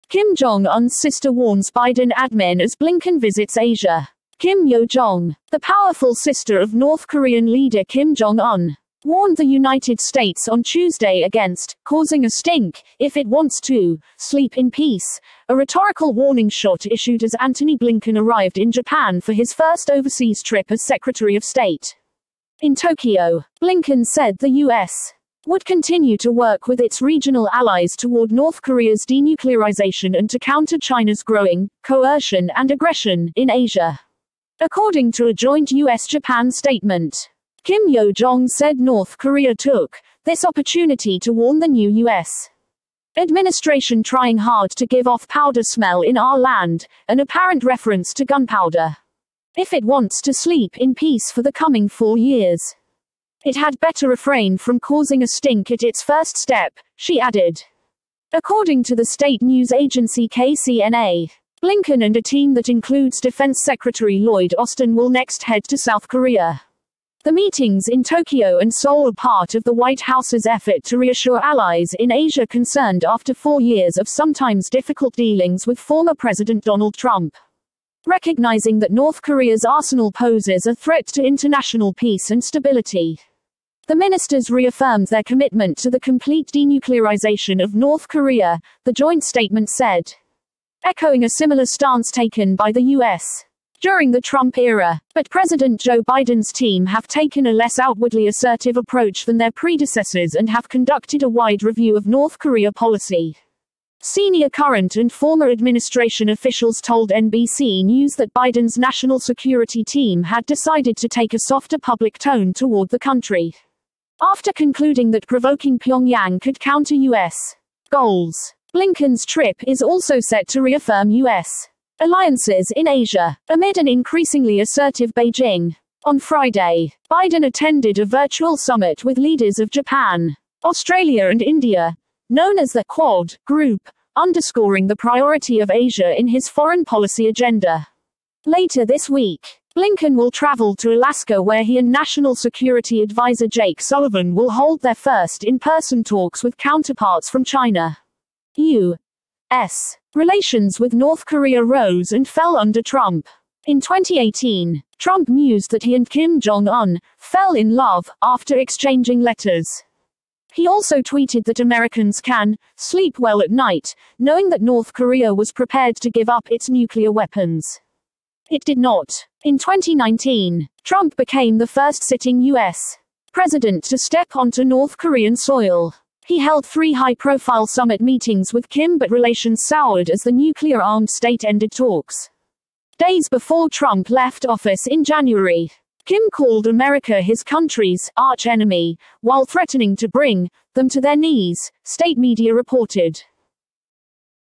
Kim Jong Un's sister warns Biden admin as Blinken visits Asia
Kim Yo Jong, the powerful sister of North Korean leader Kim Jong Un, warned the United States on Tuesday against "causing a stink" if it wants to "sleep in peace," a rhetorical warning shot issued as Antony Blinken arrived in Japan for his first overseas trip as secretary of state.

In Tokyo, Blinken said the U.S. would continue to work with its regional allies toward North Korea's denuclearization and to counter China's growing "coercion and aggression" in Asia, according to a joint U.S.-Japan statement.

Kim Yo Jong said North Korea took "this opportunity to warn the new U.S. administration trying hard to give off powder smell in our land," an apparent reference to gunpowder.

"If it wants to sleep in peace for the coming four years, it had better refrain from causing a stink at its first step," she added, according to the state news agency KCNA.

Blinken and a team that includes Defense Secretary Lloyd Austin will next head to South Korea. The meetings in Tokyo and Seoul are part of the White House's effort to reassure allies in Asia concerned after four years of sometimes difficult dealings with former President Donald Trump.

"Recognizing that North Korea's arsenal poses a threat to international peace and stability, the ministers reaffirmed their commitment to the complete denuclearization of North Korea," the joint statement said, echoing a similar stance taken by the U.S. during the Trump era.

But President Joe Biden's team have taken a less outwardly assertive approach than their predecessors and have conducted a wide review of North Korea policy. Senior current and former administration officials told NBC News that Biden's national security team had decided to take a softer public tone toward the country, after concluding that provoking Pyongyang could counter U.S. goals.

Blinken's trip is also set to reaffirm U.S. alliances in Asia, amid an increasingly assertive Beijing.

On Friday, Biden attended a virtual summit with leaders of Japan, Australia and India — known as the "Quad" group — underscoring the priority of Asia in his foreign policy agenda.

Later this week, Blinken will travel to Alaska where he and national security adviser Jake Sullivan will hold their first in-person talks with counterparts from China.

U.S. relations with North Korea rose and fell under Trump. In 2018, Trump mused that he and Kim Jong Un "fell in love" after exchanging letters. He also tweeted that Americans can "sleep well at night" knowing that North Korea was prepared to give up its nuclear weapons. It did not.

In 2019, Trump became the first sitting U.S. president to step onto North Korean soil. He held three high-profile summit meetings with Kim but relations soured as the nuclear-armed state ended talks.

Days before Trump left office in January, Kim called America his country's "arch-enemy," while threatening to bring "them to their knees," state media reported.